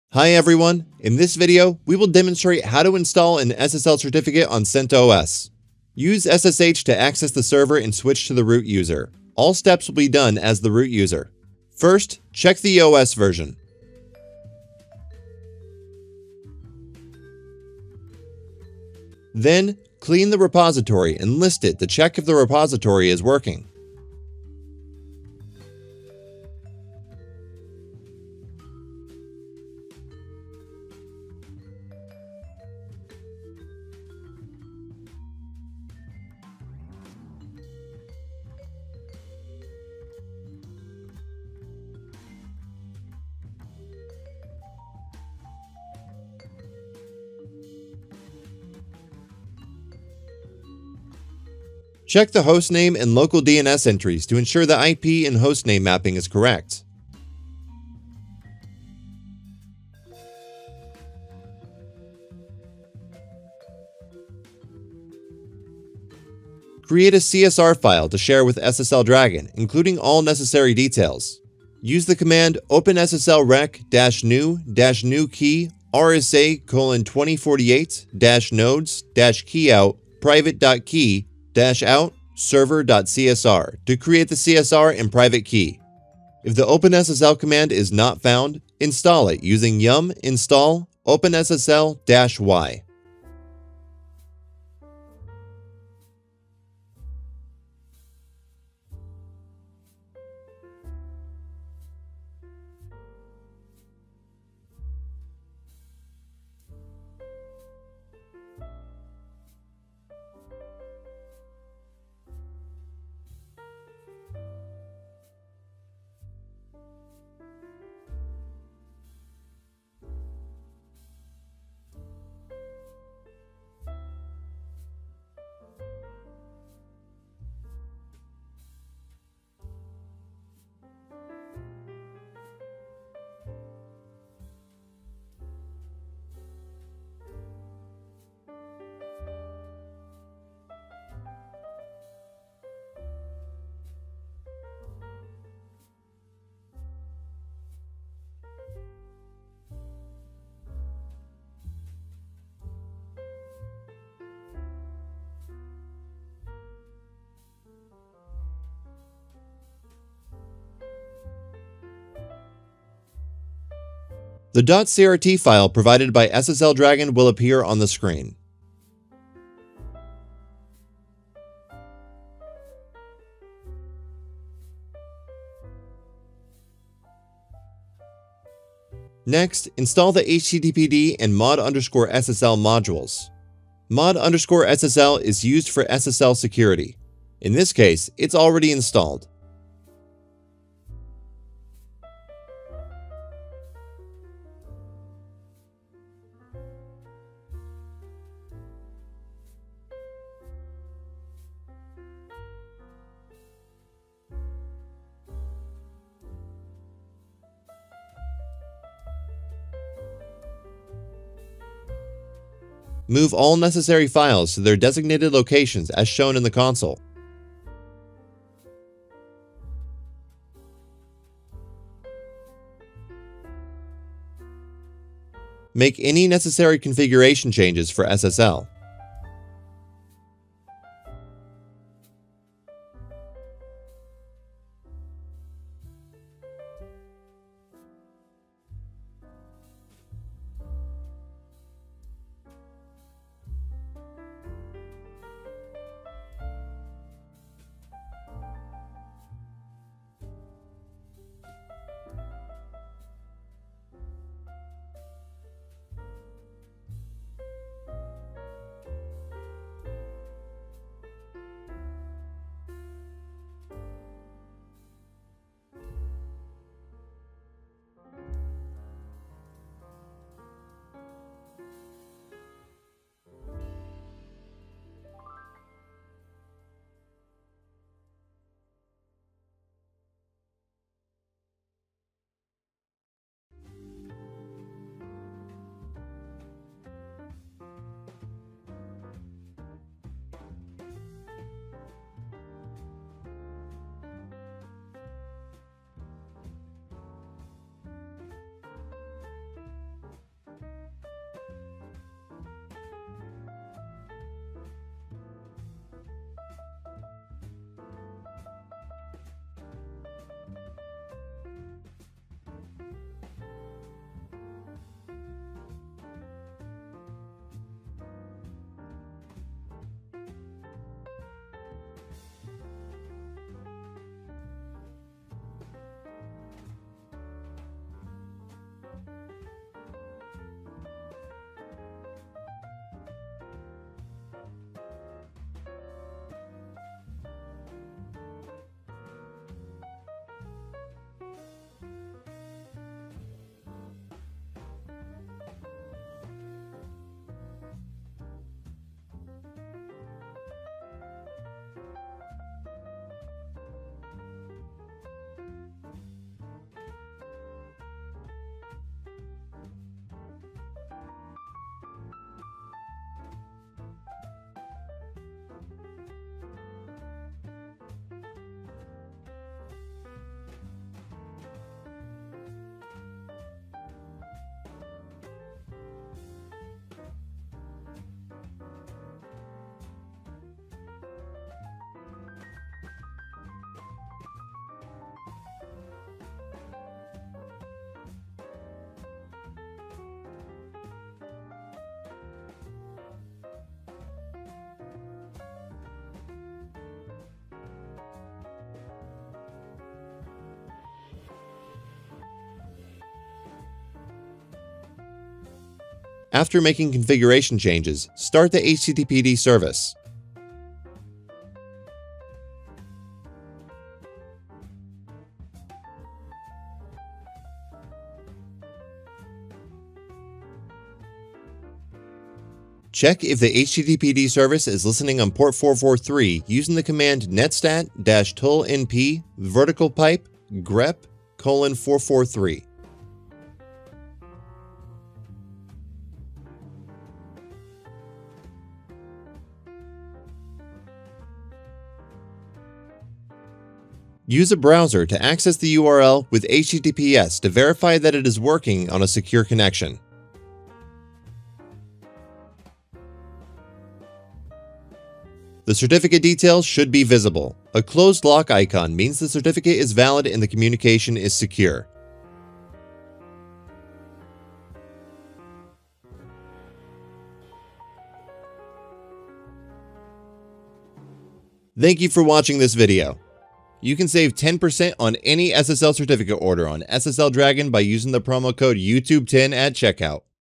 How to install an SSL Certificate on CentOS | SSL Dragon
The commands used in this video:

Fill in [ ] with your variables/file names. All commands are done with root privileges.

1. cat /etc/centos-release
2. yum clean all
3. yum repolist
4. hostname
5. cat /etc/hosts
6. openssl req -new -newkey rsa:2048 -nodes -keyout [private-key].key -out [server-name].csr
If OpenSSL isn't installed, or the command isn't found: yum install openssl -y
7. yum install httpd mod_ssl
8. Move the certificate files in their designated folders by using the following command:
mv [file] [designated location/folder]
9. vi /etc/httpd/conf.d/ssl.conf
10. systemctl start httpd
11. systemctl status httpd
12. netstat -tulpn | grep 443